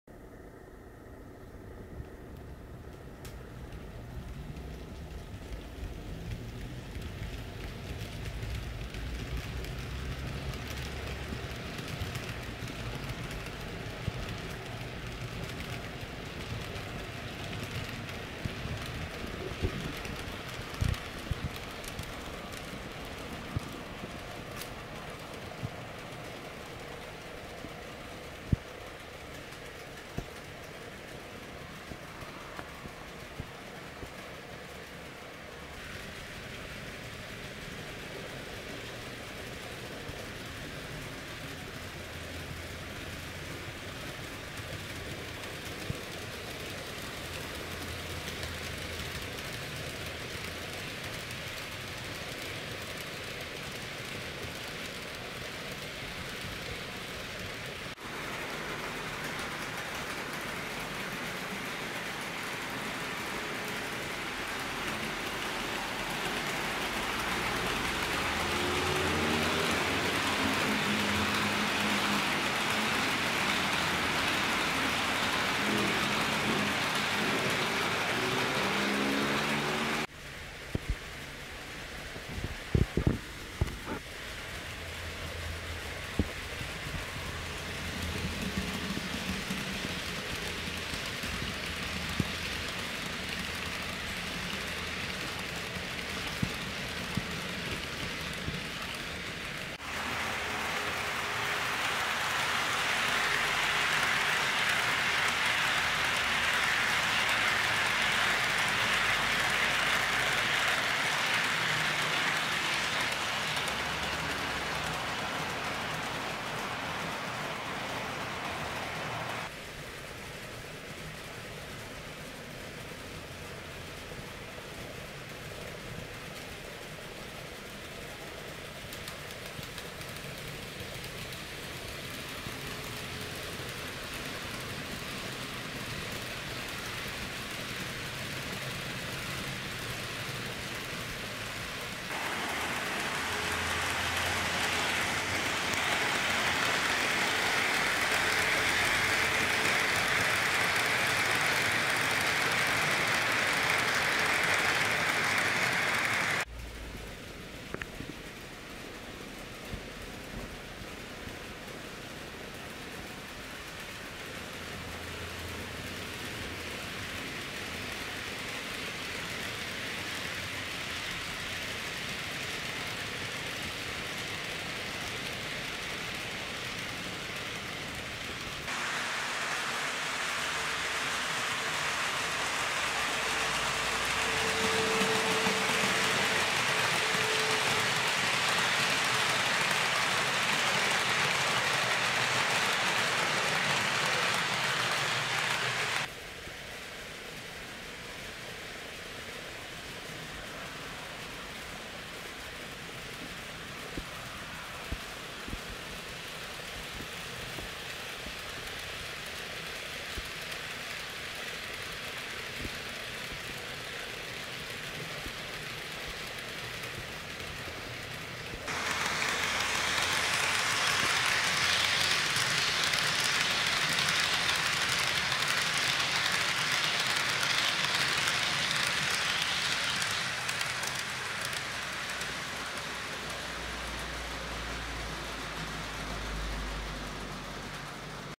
【16番(HO)】EH200牽引・タンク車編成
中央東線でのタンク車牽引をイメージしています。個人的にはEF64重連のほうが好きなのですが、現代のEH200もなかなか良いです。
【登場車両】
EH200（トミックス）
タキ43000（KATO）×9
タキ1000（KATO）×2